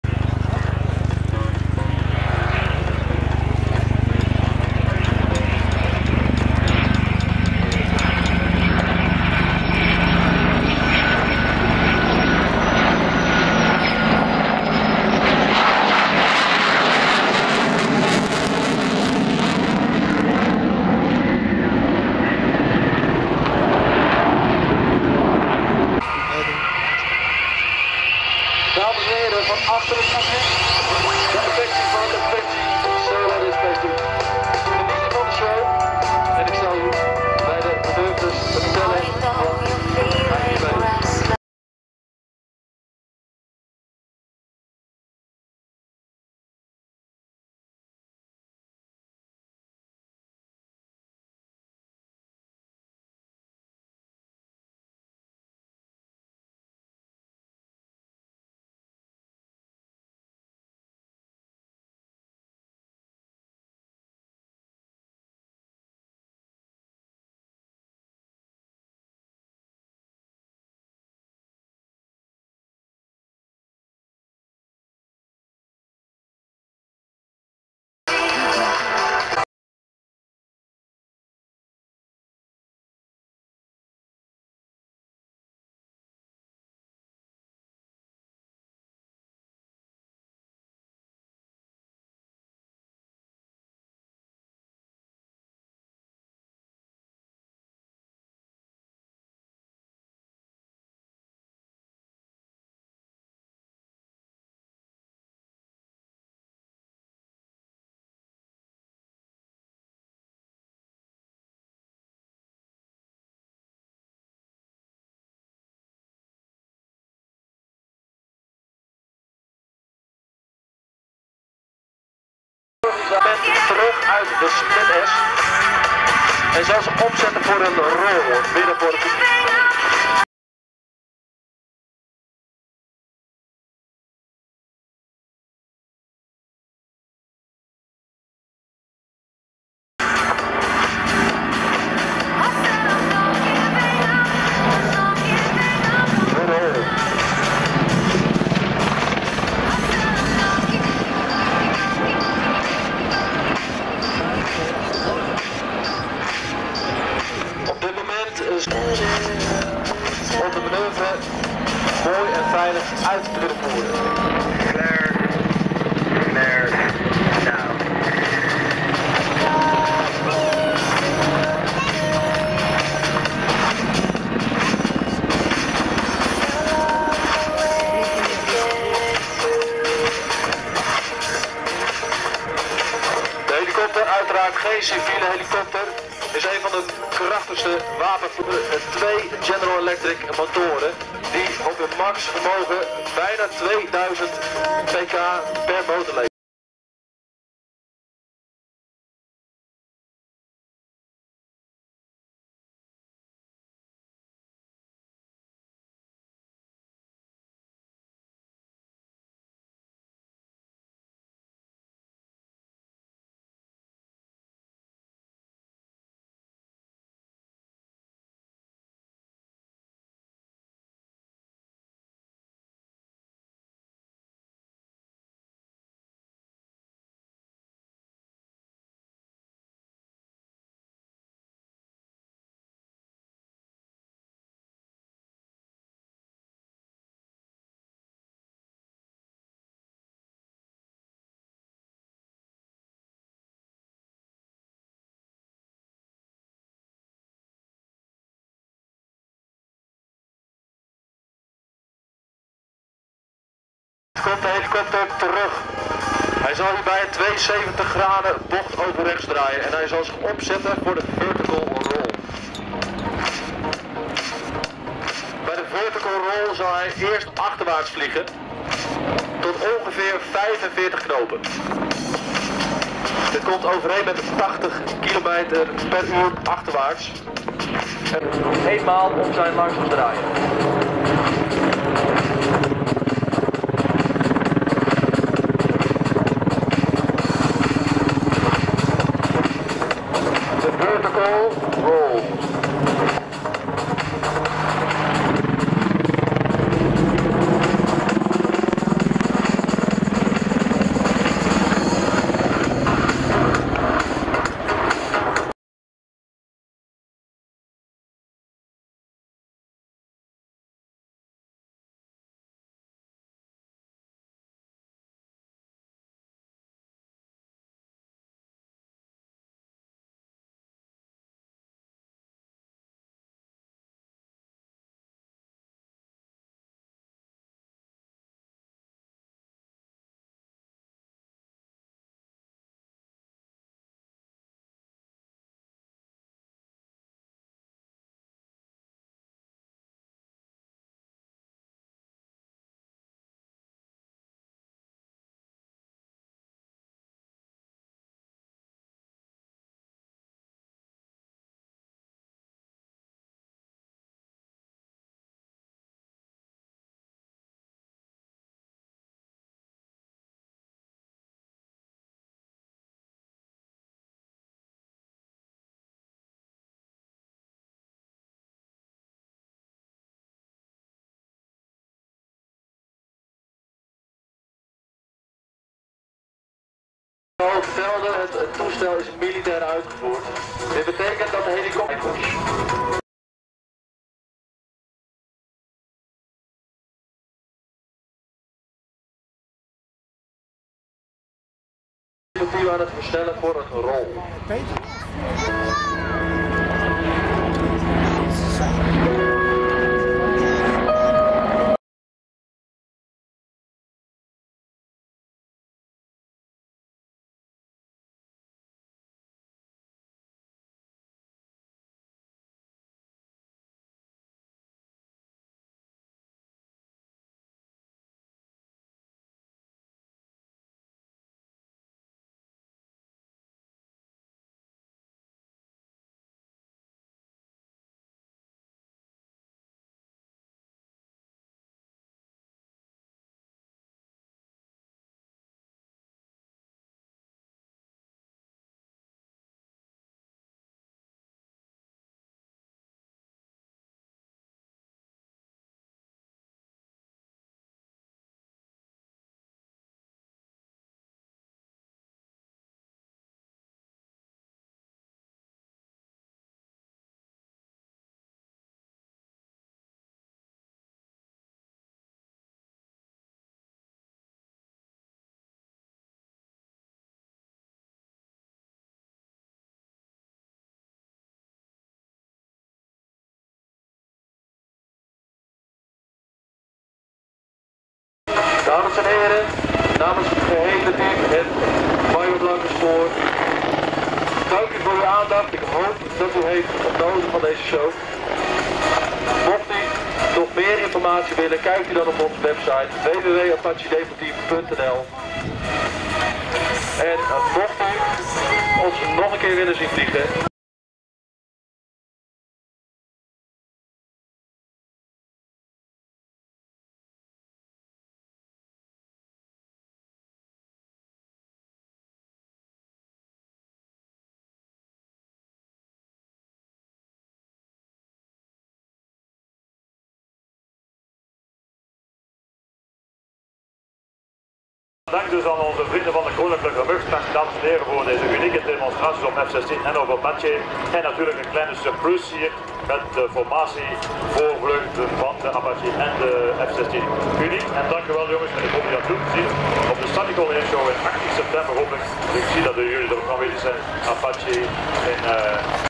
2002 Rotor Wings /// RNLAF Apache AH -64 Demoteam
apache demoteam since 2002

footage koksijde airshow 2011